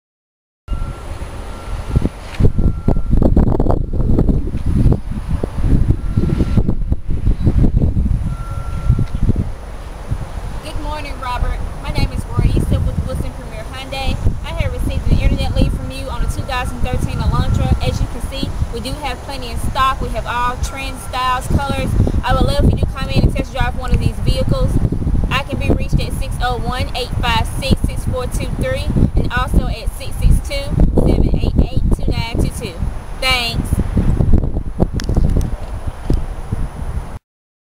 Hyundai Elantra at Wilson Premier Hyundai in Ridgeland, MS near Jackson
The Hyundai Elantra is available at Wilson Premier Hyundai in Ridgeland, MS just north of Jackson.  We have the best selection, The best Service, and Americas best warranty.